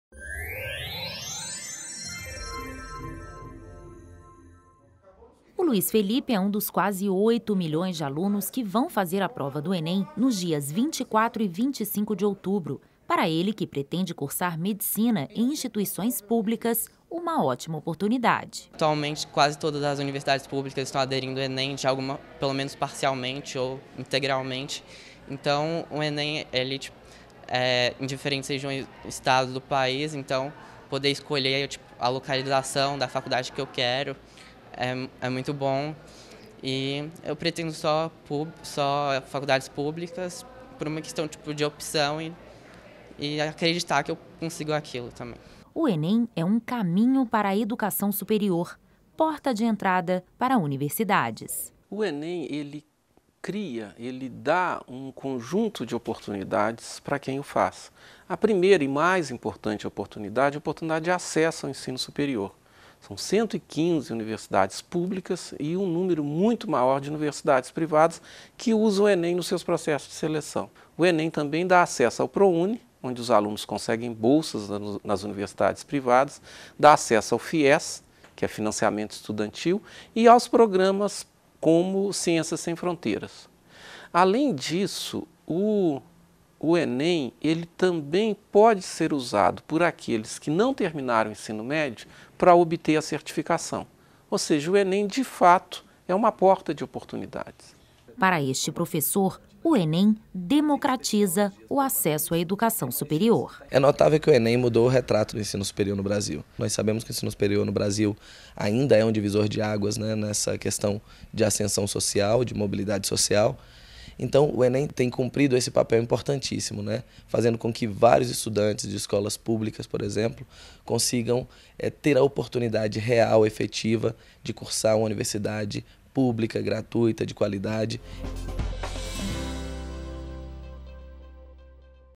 Enem 2015 - Oportunidades
Para muitos brasileiros chegar ao ensino superior é um sonho. Desde que o Governo Federal começou a usar as notas do Exame Nacional do Ensino Médio como acesso a programas que levam à graduação e ao ensino técnico, o Brasil tem aumentado significativamente o número de pessoas com ensino superior, tornando o que antes era apenas um sonho em realidade.
 Enviado às 14:17 de quarta-feira.